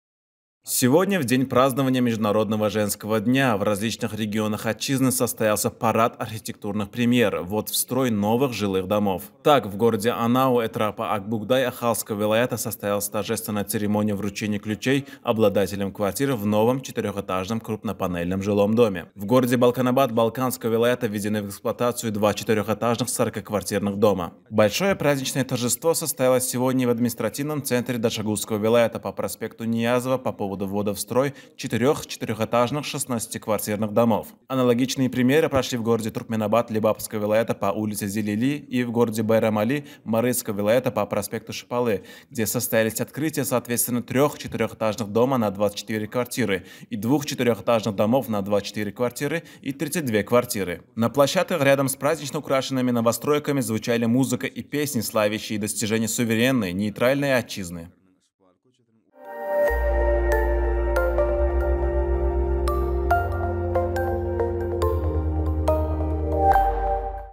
В Туркменистане прошли торжественная открытия жилых домов в честь 8 марта
8 марта, в день празднования Международного женского дня, в различных регионах Отчизны состоялся парад архитектурных премьер – ввод в строй новых жилых домов. Об этом сообщил МИЦ.

Так, в городе Анау этрапа Ак бугдай Ахалского велаята состоялась торжественная церемония вручения ключей обладателям квартир в новом 4-х этажном крупнопанельном жилом доме.

В городе Балканабат Балканского велаята введены в эксплуатацию два 4-этажных 40-квартирных дома.

Большое праздничное торжество состоялось сегодня и в административном центре Дашогузского велаята по проспекту А.Ниязова по поводу ввода в строй четырёх 4-этажных 16-квартирных домов.

Аналогичные премьеры прошли в городе Туркменабат Лебапского велаята по улице Зелили и городе Байрамали Марыйского велаята по проспекту Шыпалы, где состоялись открытия, соответственно, трёх 4-этажных дома на 24 квартиры и двух 4-этажных домов на 24 квартиры и 32 квартиры.

На площадках рядом с празднично украшенными новостройками звучали музыка и песни, славящие достижения суверенной нейтральной Отчизны.